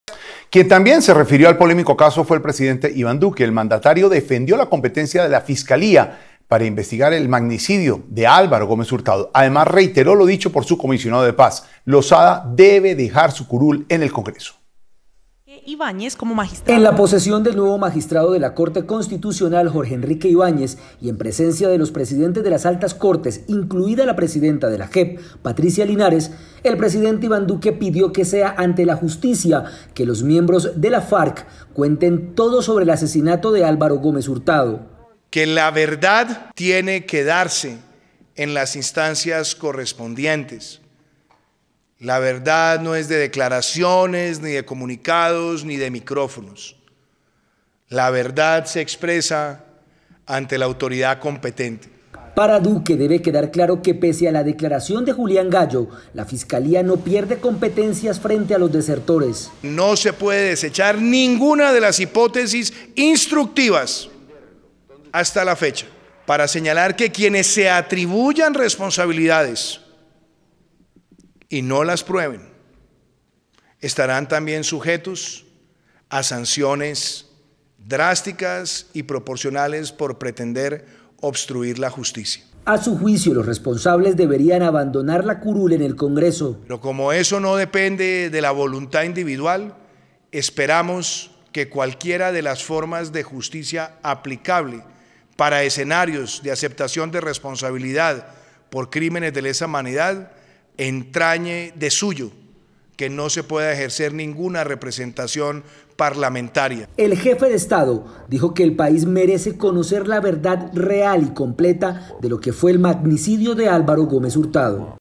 Duque dice que Carlos Antonio Lozada debe renunciar al Congreso por magnicidio de Álvaro Gómez
El presidente indicó, en presencia de los magistrados de las Altas Cortes, que la Fiscalía es quien debe investigar el asesinato del líder conservador.